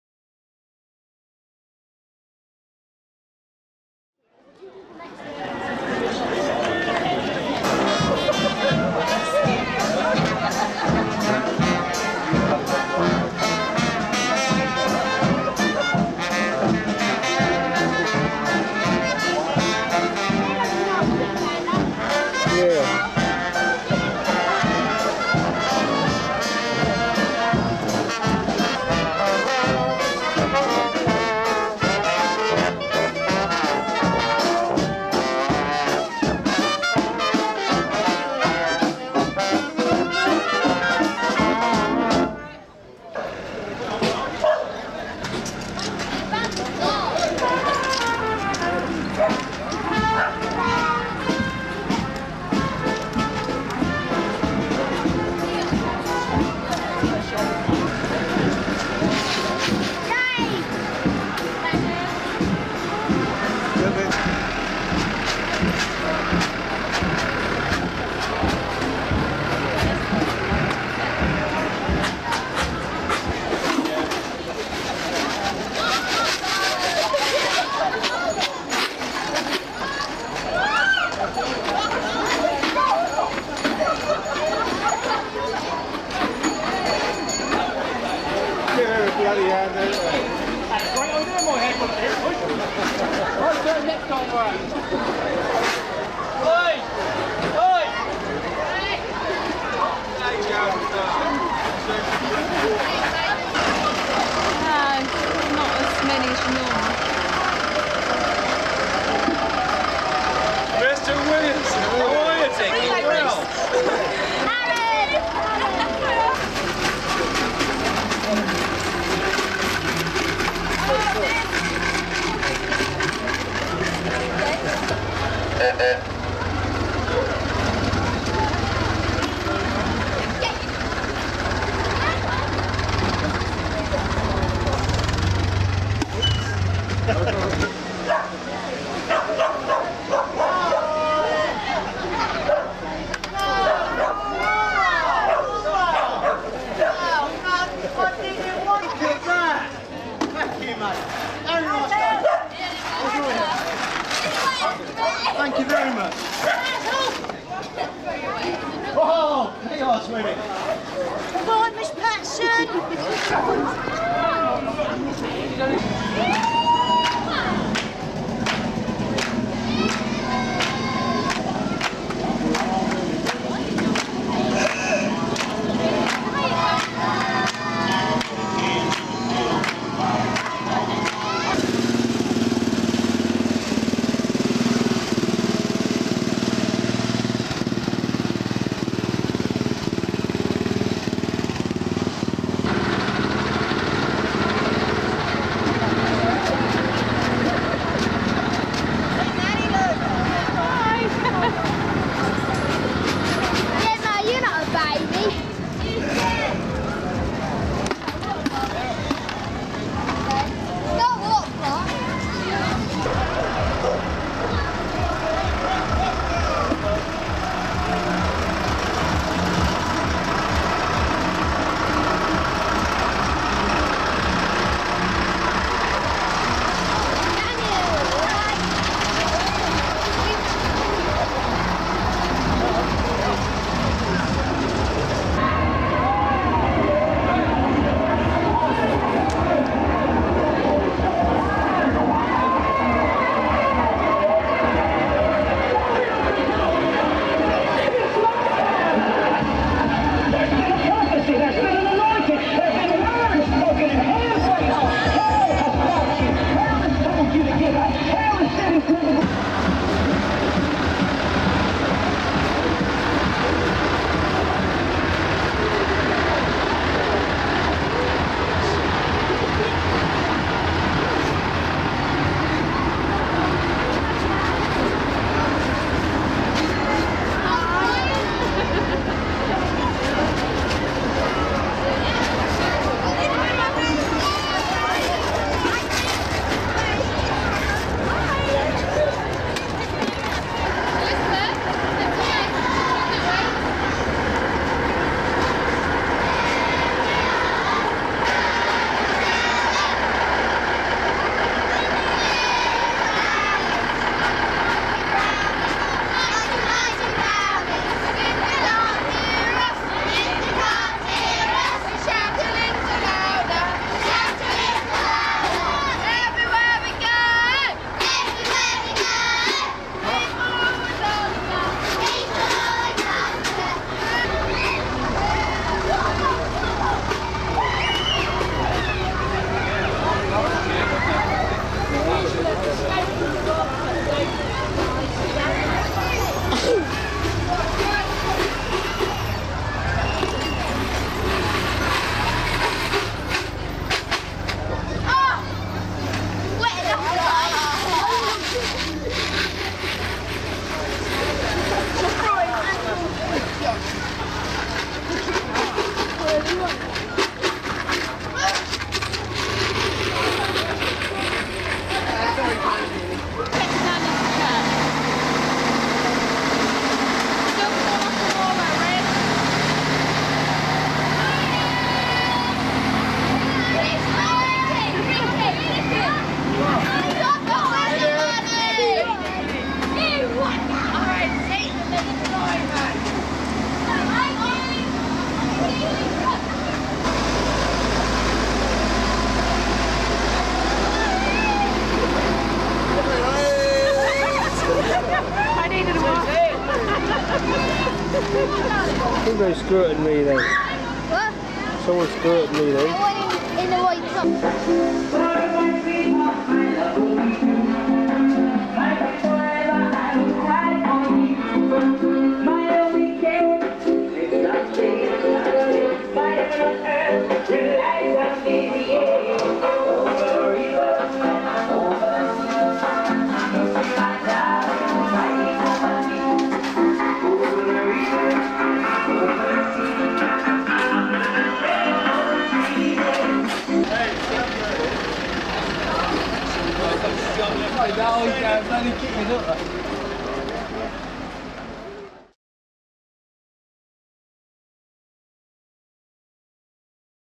Wells Carnival, Norfolk - 1998 Wells Carnival Parade, Wells-next-the-Sea, Norfolk
Many thanks to Brian Frary for allowing us to use the video he filmed of the Wells Carnival Parade back in 1998, as part of our Virtual Carnival 2020.